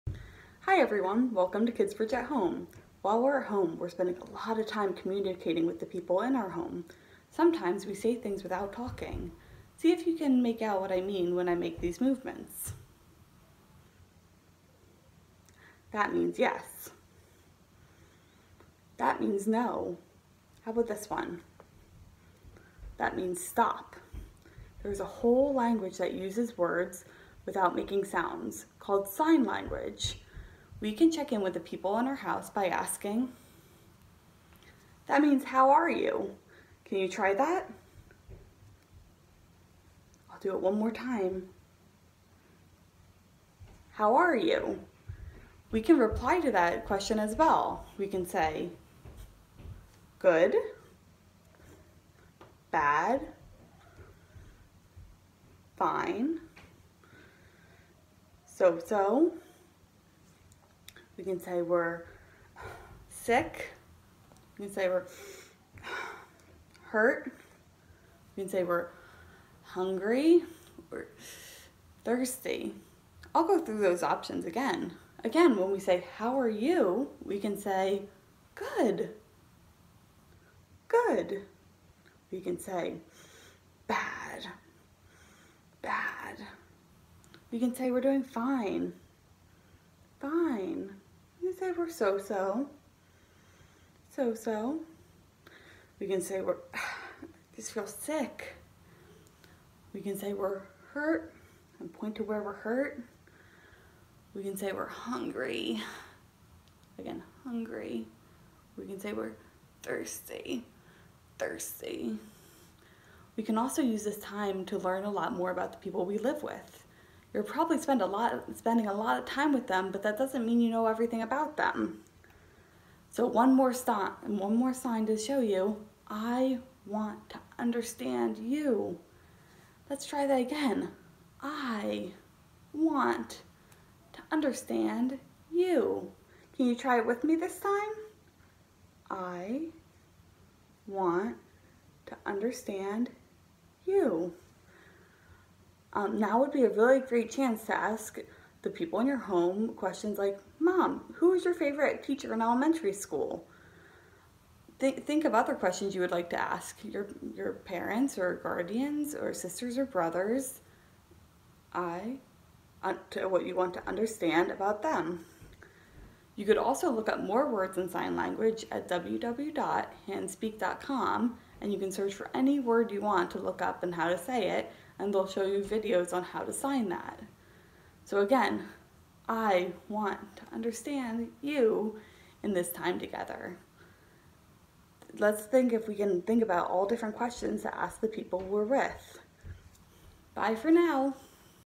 American Sign Language
Paige, a trained Kidsbridge Tolerance Center facilitator, presents several phrases in American Sign Language to expand how families can communicate about their feelings.